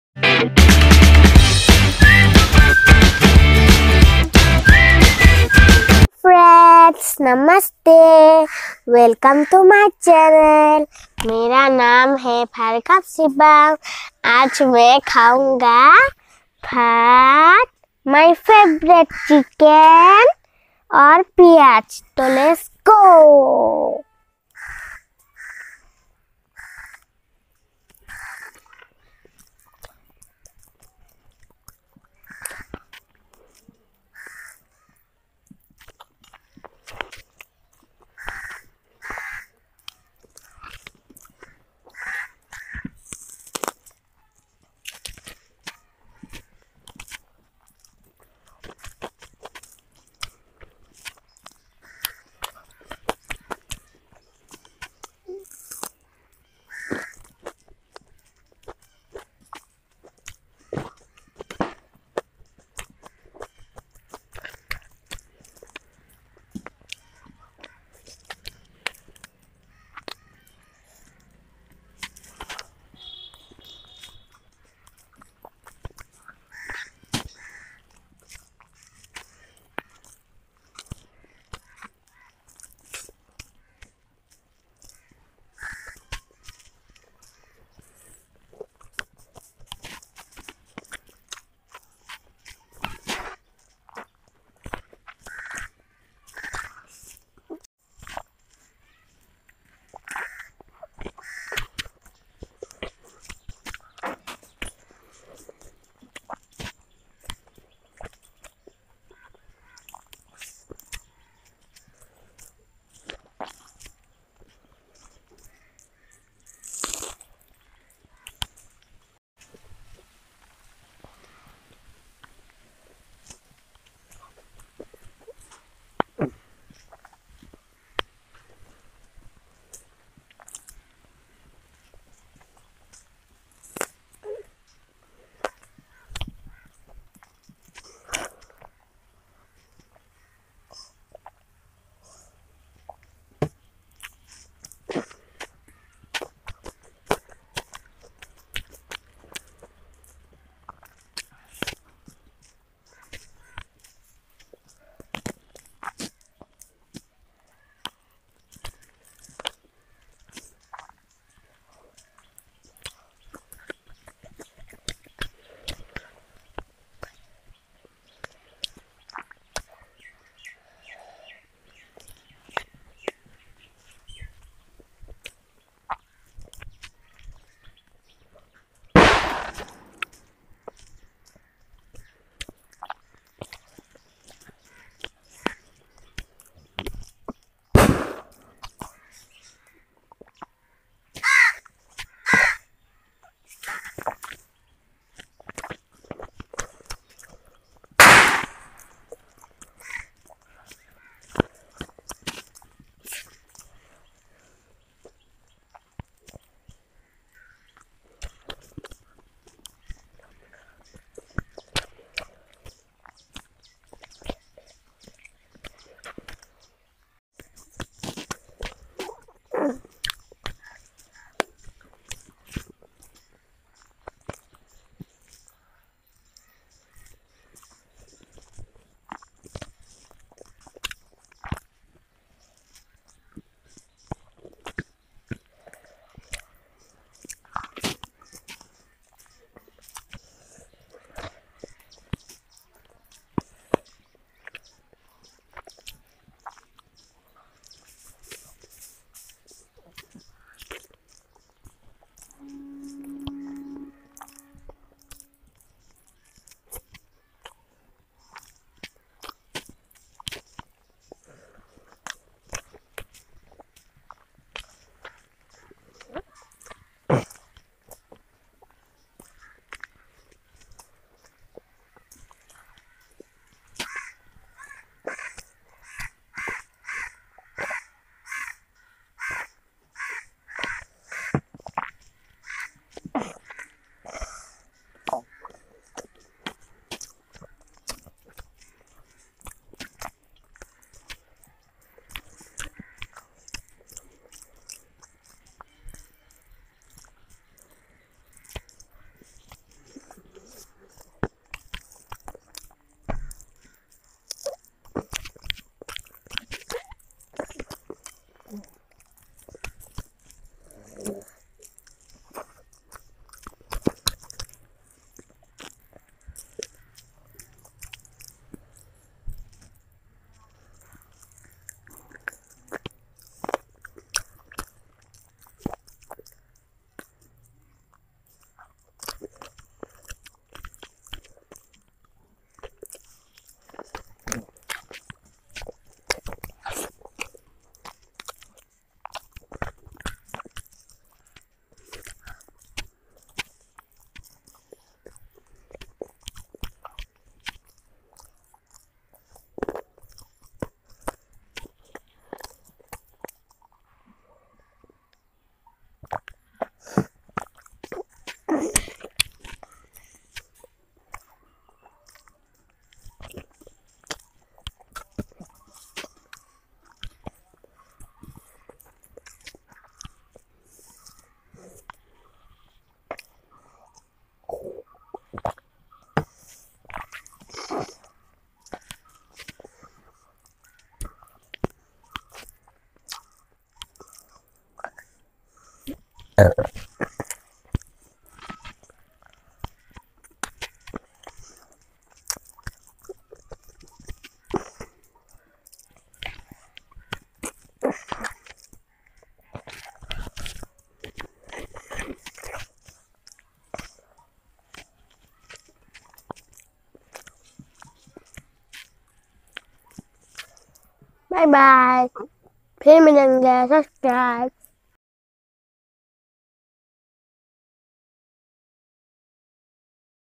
Bhaat aur broiler chicken se kiya pet puja
Aaj ka khana special hai kyuki aaj mere lunch mein chicken hai. Bohut hi tasty chicken banaya mummy ne.

Phir milenge friends tab tak ke liye bye bye